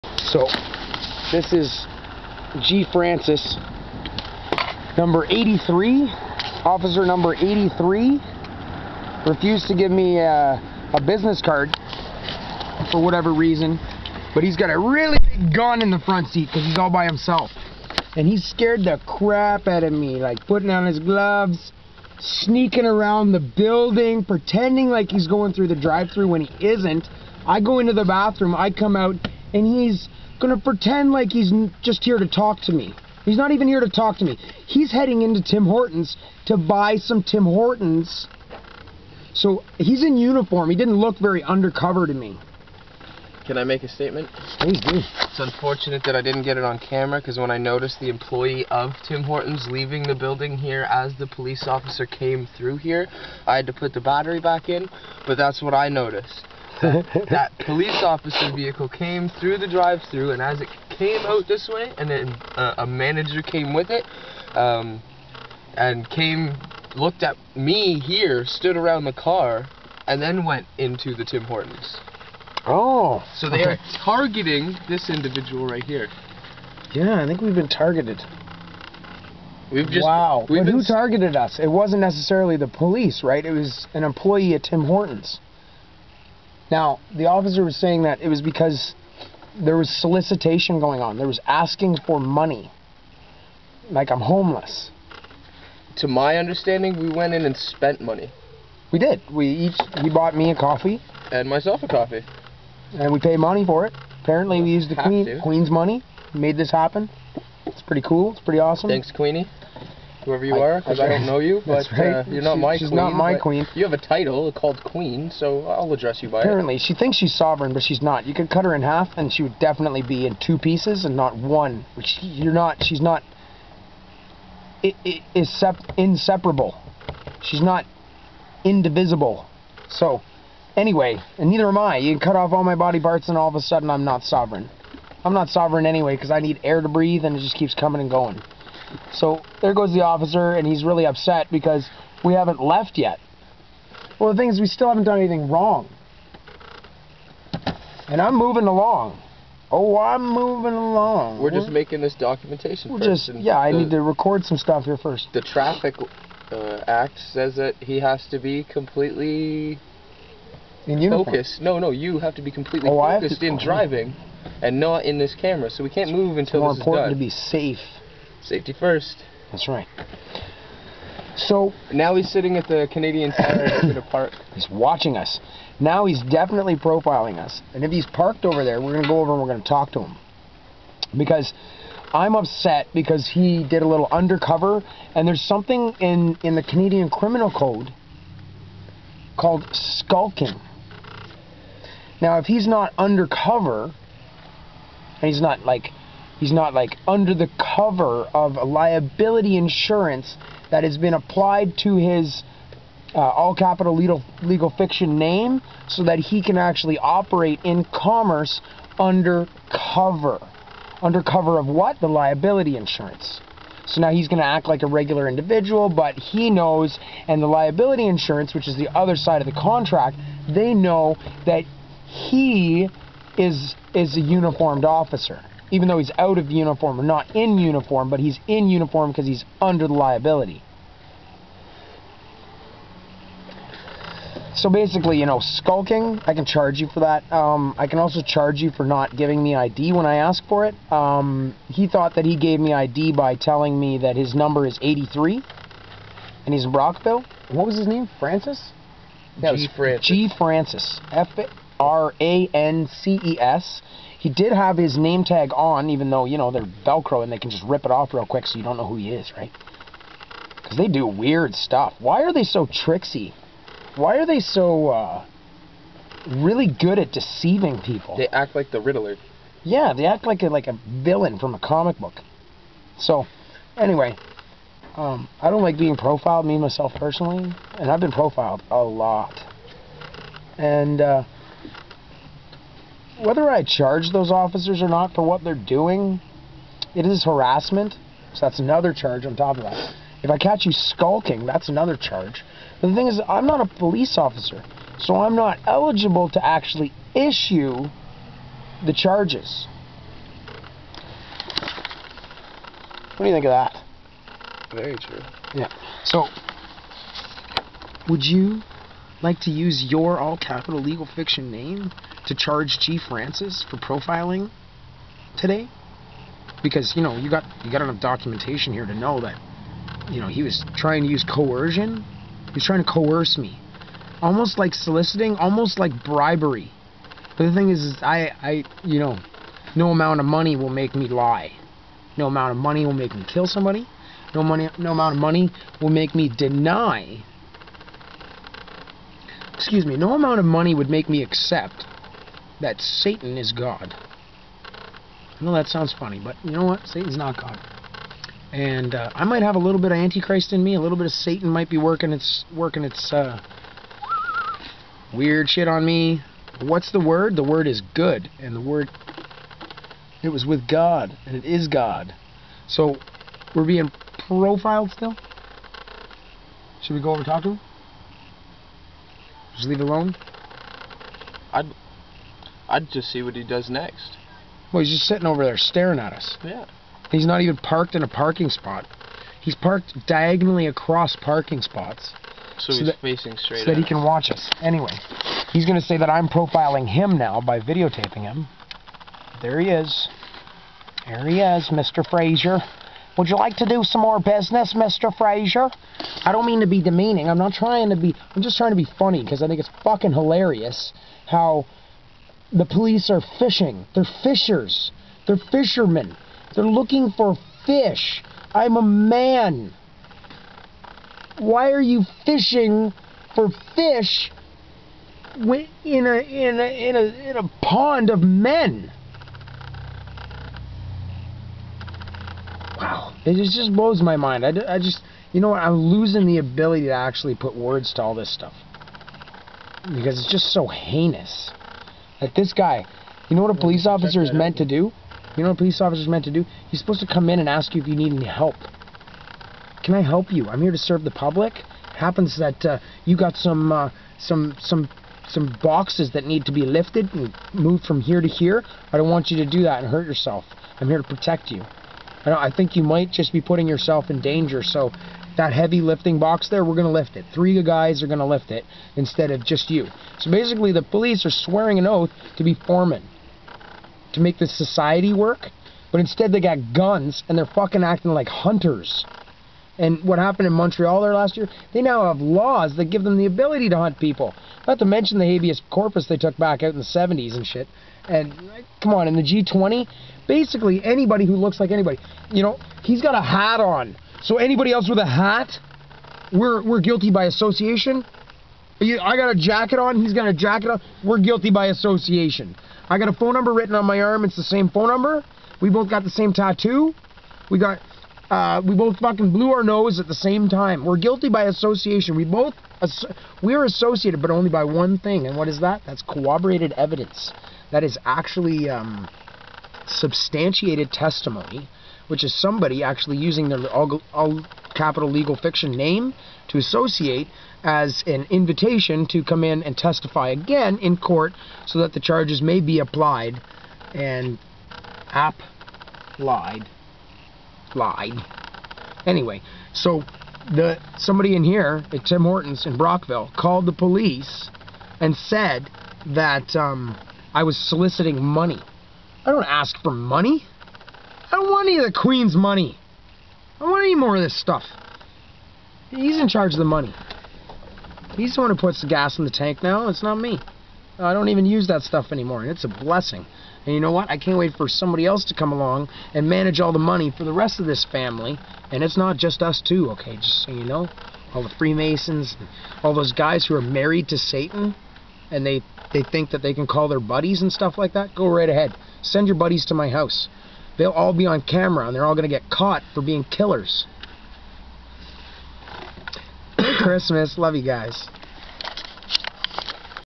G.FRANCES Police Officering c/o GrasshopperMedia.ca
Brockville Ontario police harassment and intimidation caught on tape.

Gotta come up with a better lie than "he was soliciting and asking for change"...silly workers...I want real change...will buy money if you think change is money. I have too much to spend at the moment. If you need help though, just ask. His semi-automatic was bigger than my leg.